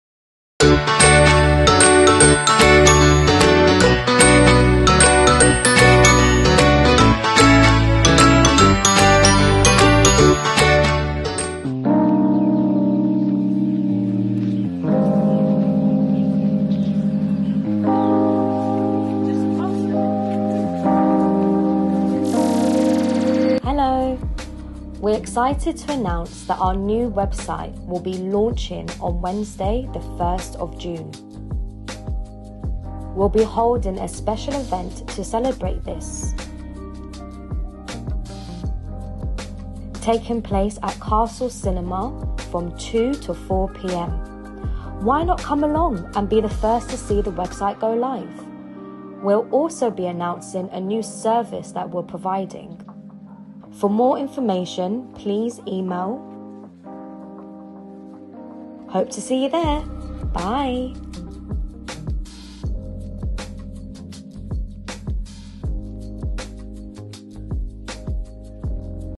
Sign for All 'Website Launch on Wednesday 1st June'
We are excited to announce our new website will be launching on Wednesday 1st June, to celebrate we are hosting an event at Castle Cinema, from 2-4pm.

Why not come along and be the first to see the website go live!

We will also announce a new service along with some special treats for you to take home.